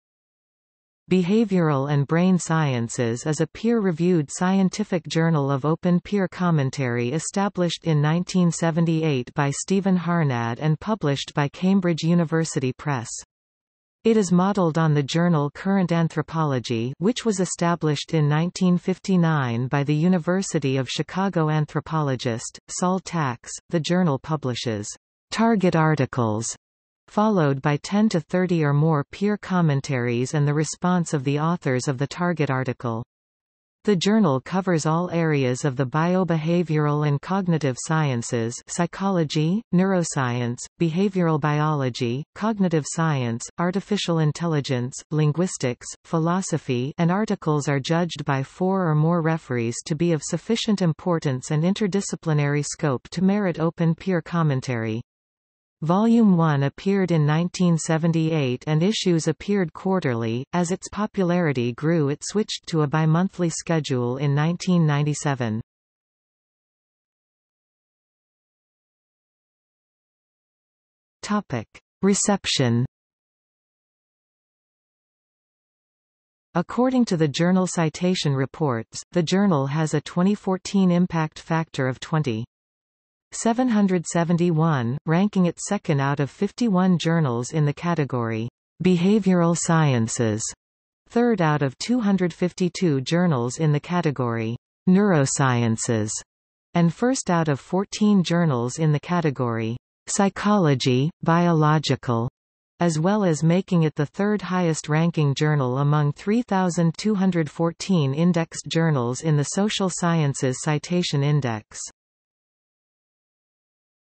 Behavioral and Brain Sciences | Wikipedia audio article
00:01:07 Reception

- Socrates

SUMMARY
Behavioral and Brain Sciences is a peer-reviewed scientific journal of Open Peer Commentary established in 1978 by Stevan Harnad and published by Cambridge University Press. It is modeled on the journal Current Anthropology (which was established in 1959 by the University of Chicago anthropologist, Sol Tax).The journal publishes "target articles" followed by 10 to 30 or more peer commentaries and the response of the authors of the target article. The journal covers all areas of the biobehavioral and cognitive sciences (psychology, neuroscience, behavioral biology, cognitive science, artificial intelligence, linguistics, philosophy) and articles are judged by four or more referees to be of sufficient importance and interdisciplinary scope to merit Open Peer Commentary. Volume 1 appeared in 1978 and issues appeared quarterly; as its popularity grew it switched to a bimonthly schedule in 1997.